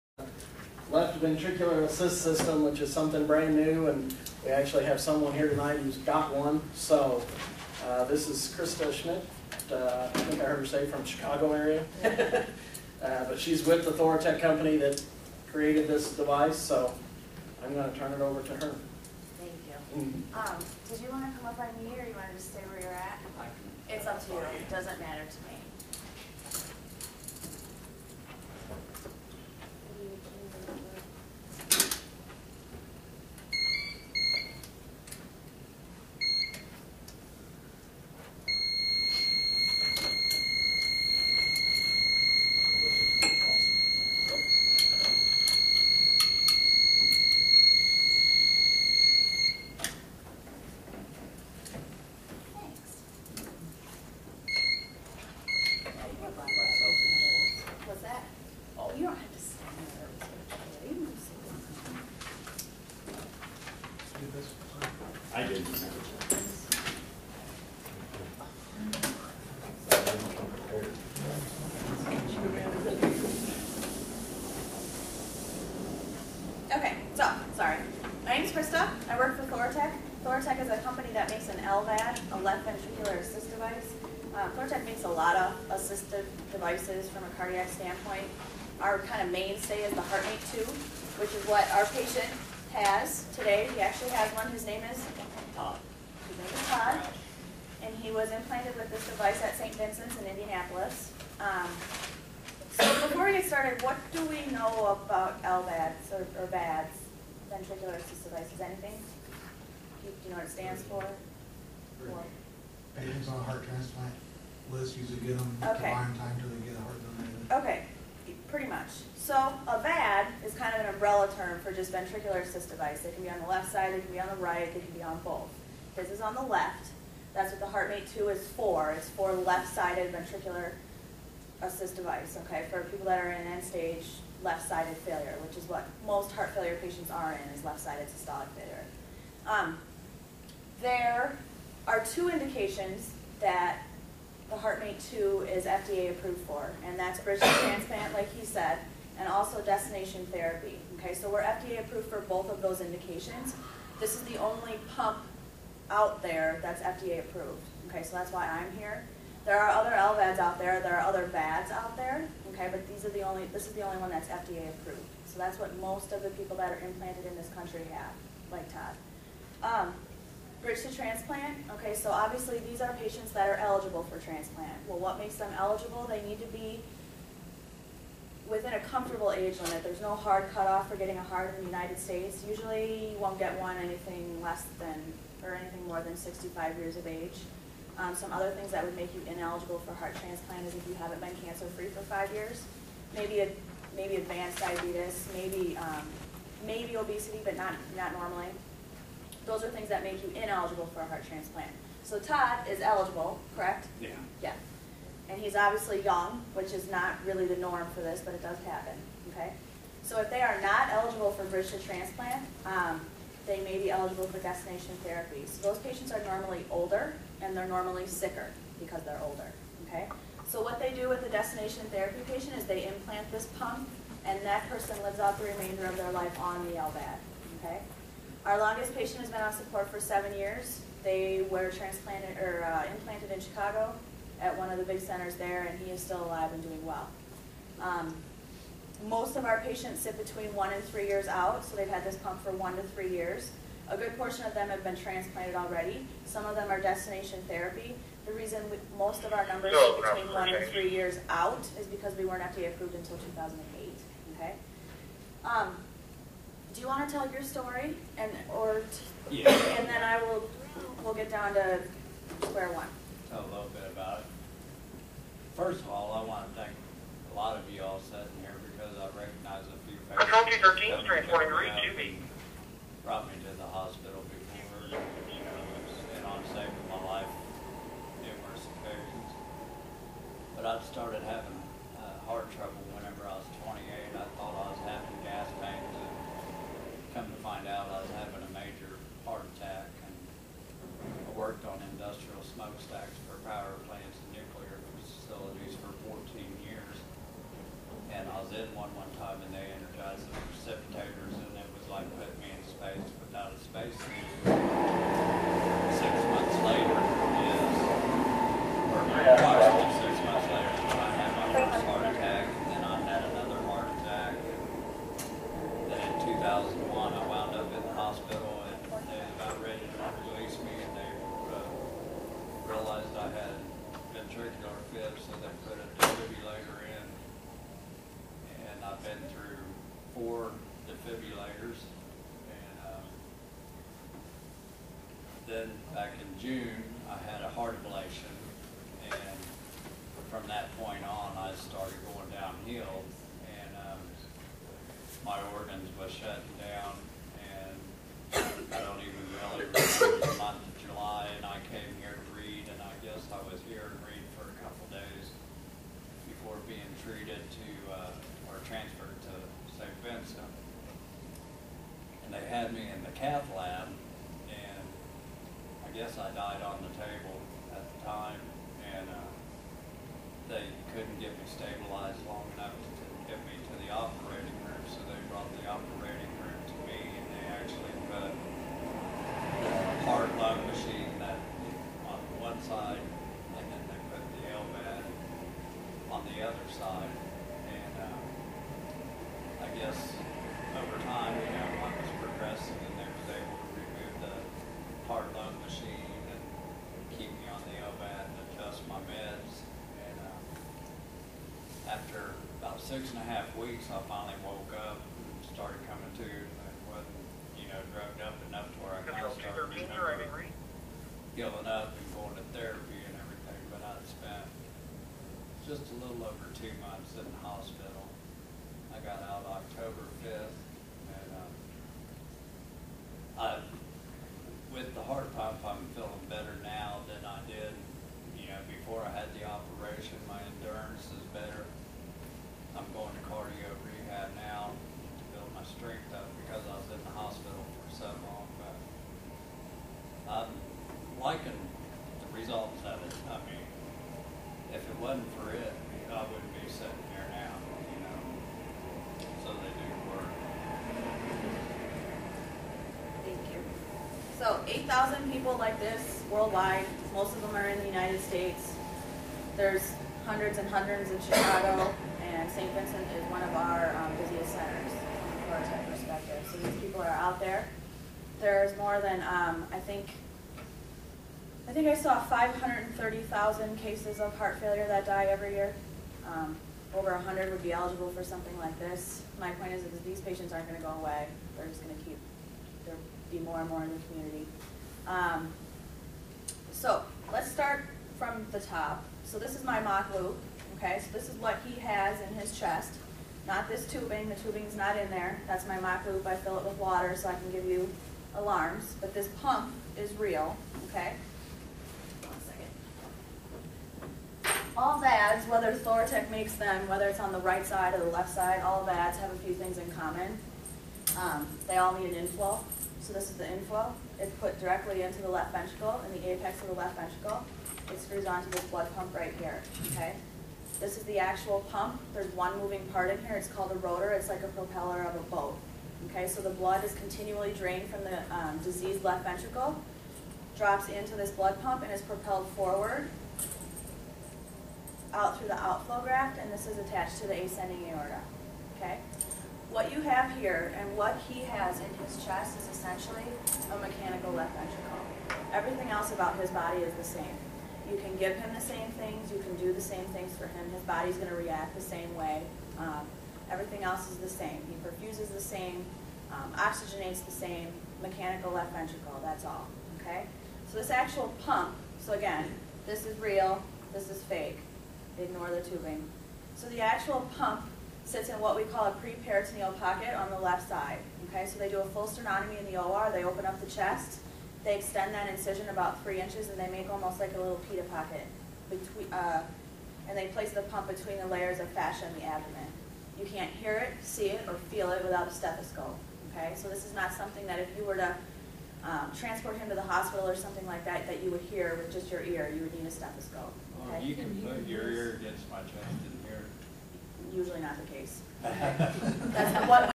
Heart Mate L-VAD training 1
Hello, This is a training class that was hosted by Rural Metro Ambulance. About the Heart Mate 2 L-VAD, made by Thoratec. It is now what I have implanted in me now to keep me alive. I have a barely functioning heart. If not for it I wouldn't be alive now. The training was for all Emergency E.M.T.s,